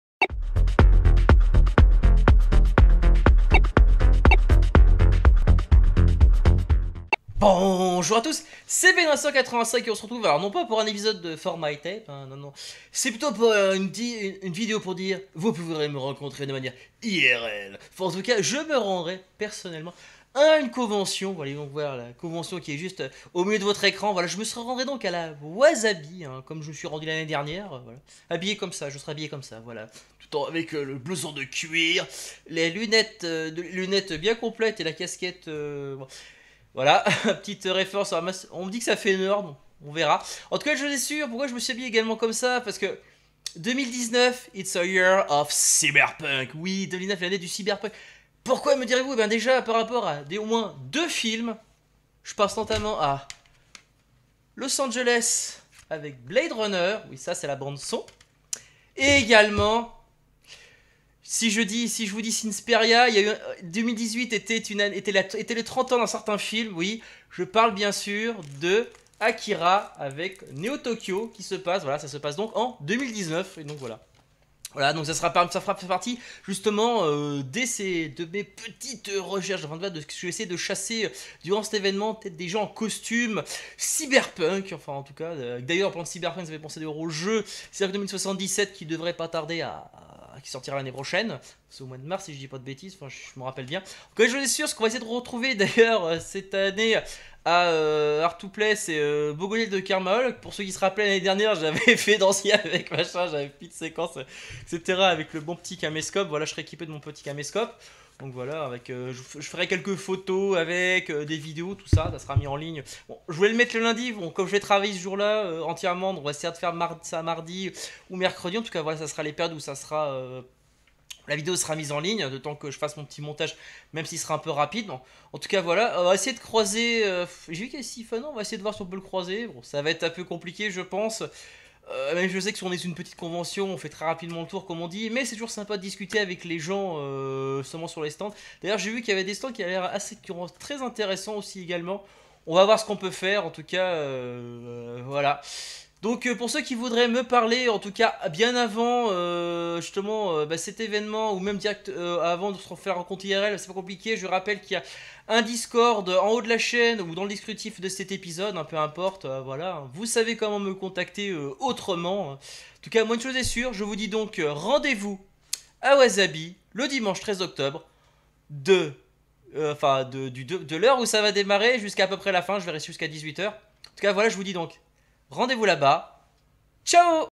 Vlog: Je me rends à la Wazabi's Odyssey (2019)
Petite vidéo annonce avec les objectifs à atteindre.
RDV le Dimanche 13 Octobre jusqu’à 18h. ;)

N'hésitez pas à liker et à commentez cette vidéo.
N'hésitez pas également à la partager avec vos amis.